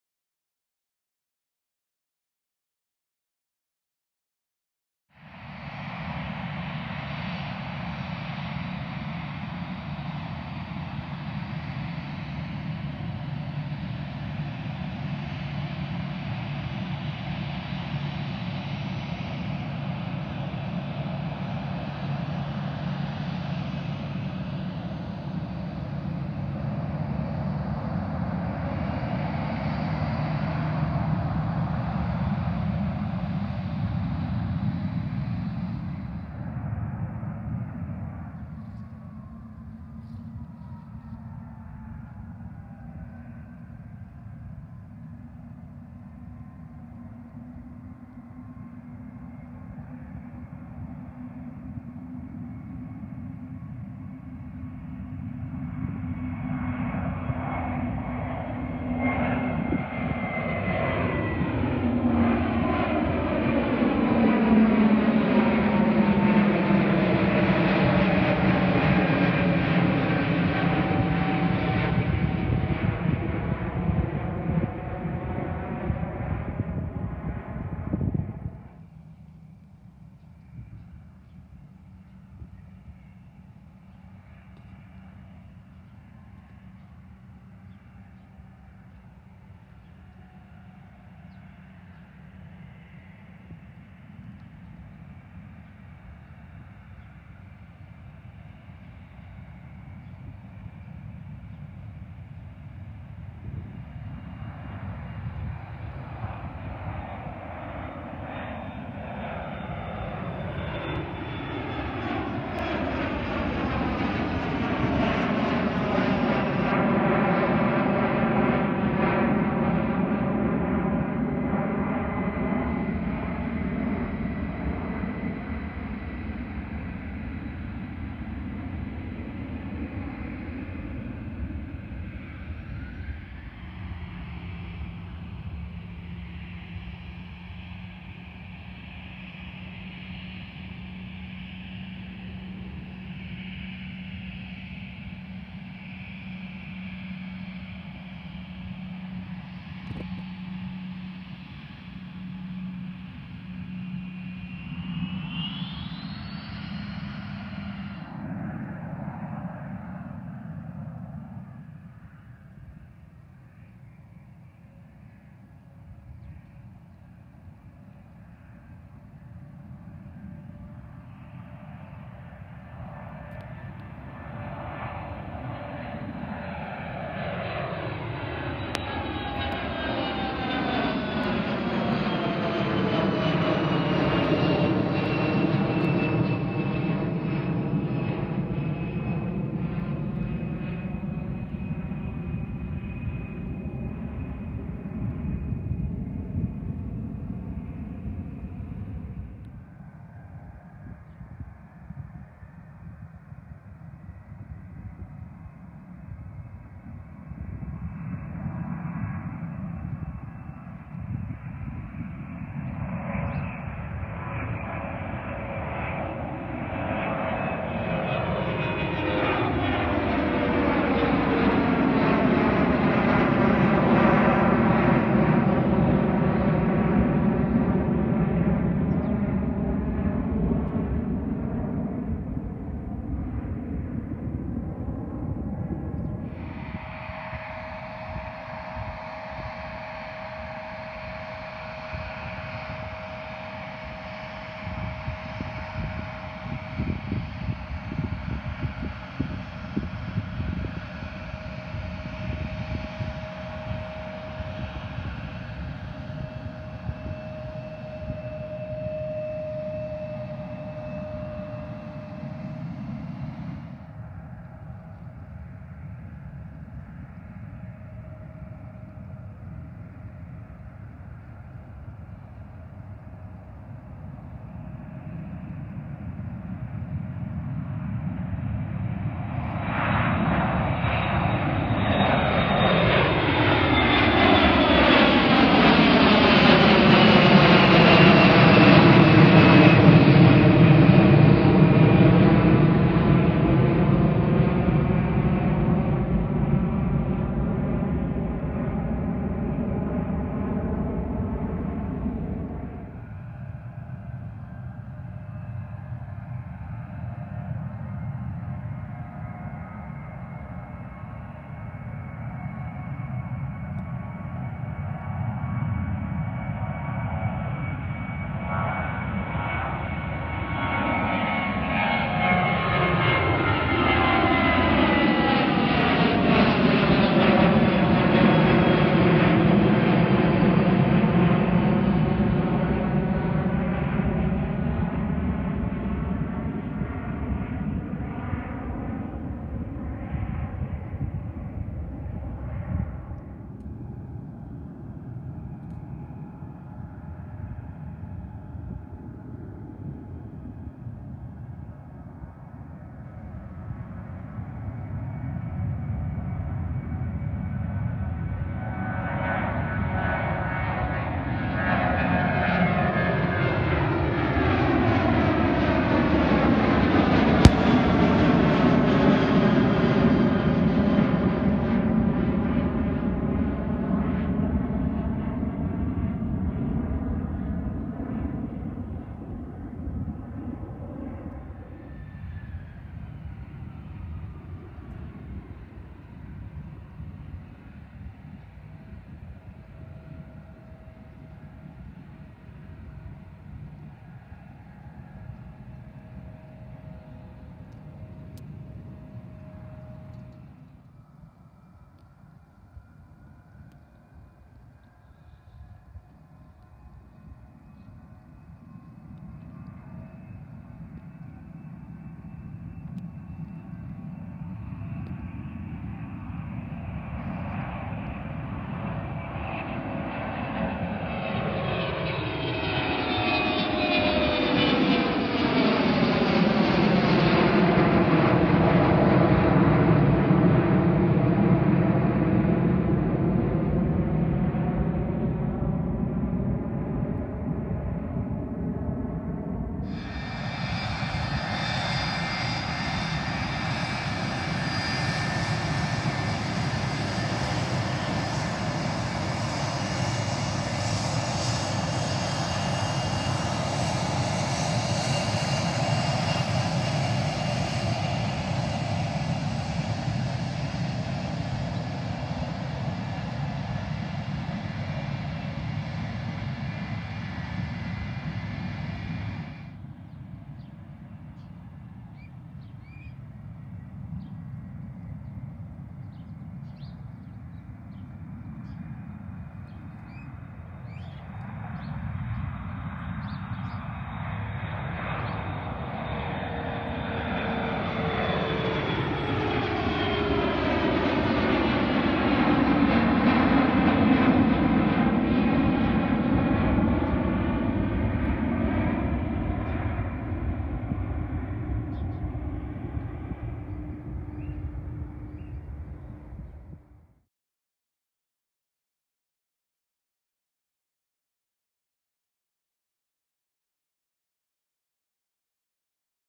New Spotting Location! | Cleveland Hopkins Plane Spotting 2019
To kick off the new year, I was finally able to spot a location that I’ve been trying to get to for a while now. I was able to catch the planes arriving and departing in both directions.  Be sure to watch parts two and three also, as the planes on the taxiway get extremely close!